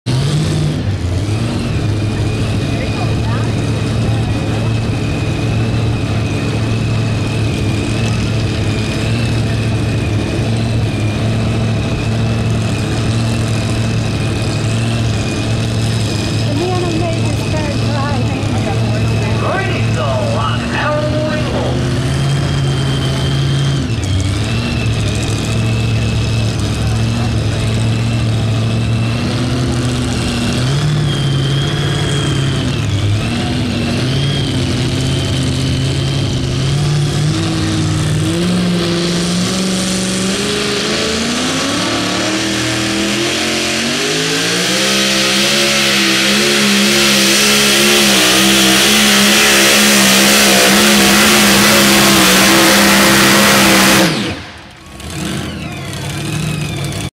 Howling Wolf SMTWD Washington county Fair 2012
Roy Dietzel Driving the Howling Wolf for the 1pm show at the Washington County Fair in Greenwich NY